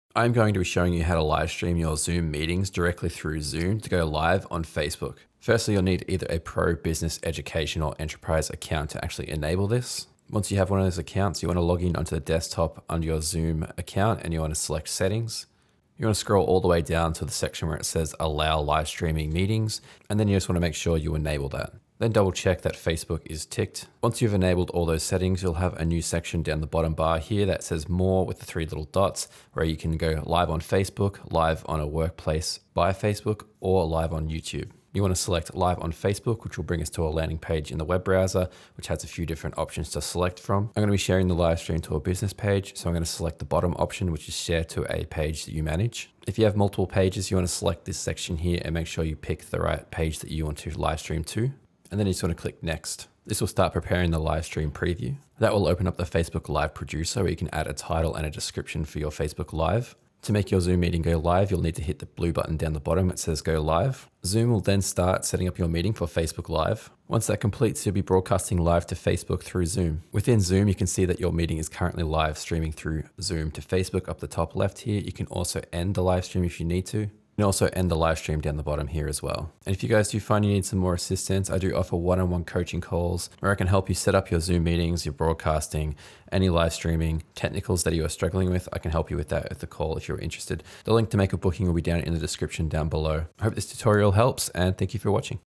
How to Live Stream Zoom on Facebook Live
how to Livestream to Facebook through zoom to help broadcast your zoom meetings or webinars.